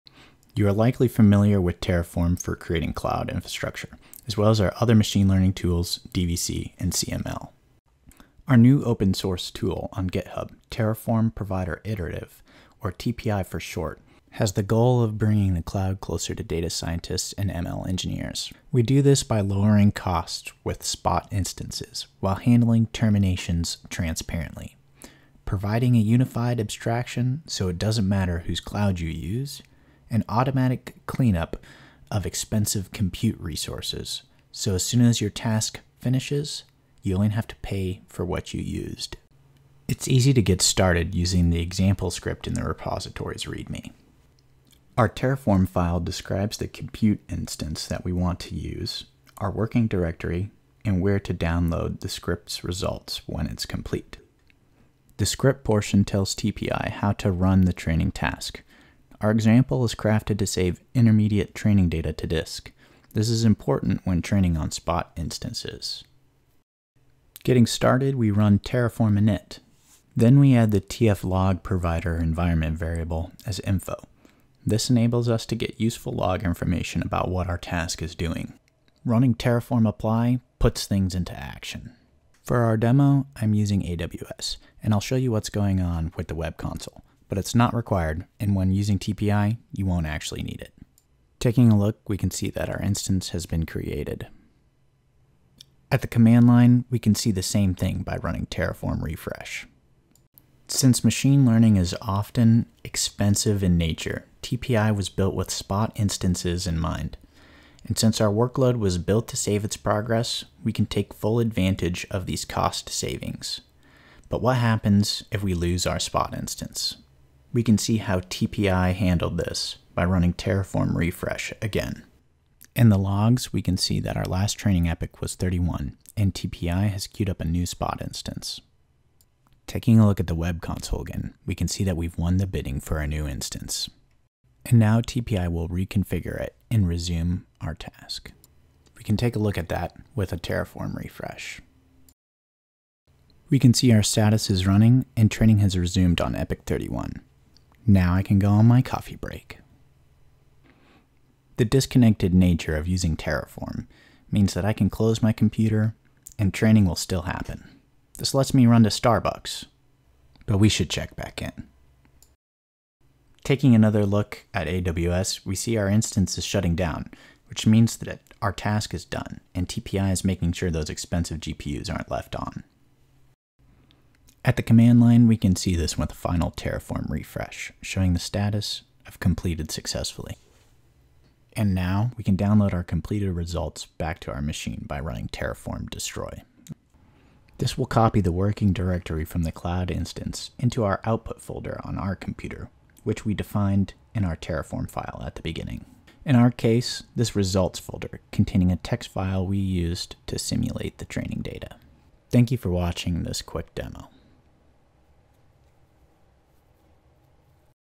Run ML projects with automatic cloud Data sync & Spot recovery: Terraform + Iterative (open source)
Terraform Provider Iterative (TPI) is a Terraform plugin built with machine learning in mind. This CLI tool offers full lifecycle management of computing resources (including GPUs and respawning spot instances) from several cloud vendors (AWS, Azure, GCP, K8s)... 𝘄𝗶𝘁𝗵𝗼𝘂𝘁 𝗻𝗲𝗲𝗱𝗶𝗻𝗴 𝘁𝗼 𝗯𝗲 𝗮 𝗰𝗹𝗼𝘂𝗱 𝗲𝘅𝗽𝗲𝗿𝘁.

💵 𝗟𝗼𝘄𝗲𝗿 𝗰𝗼𝘀𝘁 𝘄𝗶𝘁𝗵 𝘀𝗽𝗼𝘁 𝗿𝗲𝗰𝗼𝘃𝗲𝗿𝘆: transparent data checkpoint/restore & auto-respawning of low-cost spot/preemptible instances
🚫🔒 𝗡𝗼 𝗰𝗹𝗼𝘂𝗱 𝘃𝗲𝗻𝗱𝗼𝗿 𝗹𝗼𝗰𝗸-𝗶𝗻: switch between clouds with just one line thanks to unified abstraction
🚫🗑 𝗡𝗼 𝘄𝗮𝘀𝘁𝗲: auto-cleanup unused resources (terminate compute instances upon task completion/failure & remove storage upon download of results), pay only for what you use
🧑🏽‍💻 𝗗𝗲𝘃𝗲𝗹𝗼𝗽𝗲𝗿-𝗳𝗶𝗿𝘀𝘁 𝗲𝘅𝗽𝗲𝗿𝗶𝗲𝗻𝗰𝗲: one-command data sync & code execution with no external server, making the cloud feel like a laptop
Supported cloud vendors include:
- Amazon Web Services
- Microsoft Azure
- Google Cloud Platform
- Kubernetes

0:00 Intro
0:18 Features
0:40 Get started - main.tf
1:06 terraform init - installing dependencies
1:18 terraform apply - provisioning cloud resources & uploading task
1:31 Understanding what’s going on outside of the command line
1:35 terraform refresh
1:40 Auto-recovering spot instances
2:23 Close your laptop - time for a break
2:44 Back online, checking in
3:00 terraform destroy - downloading results & cleaning up
3:22 Thanks for watching!